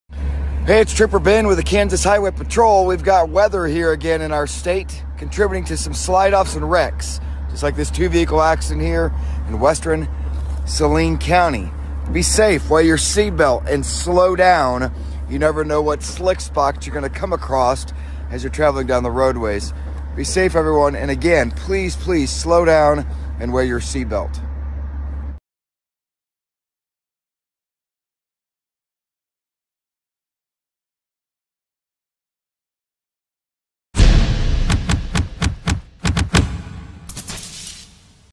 Icy Kansas Crash! Semi-Trailer Hangs Off Frozen Highway Bridge
Footage from the Kansas Highway Patrol shows a trooper giving the public a warning to drive carefully in icy situations as he showed a Fedex semi-trailer hanging off a bridge after crashing into another semi-truck. No injuries were reported in the incident.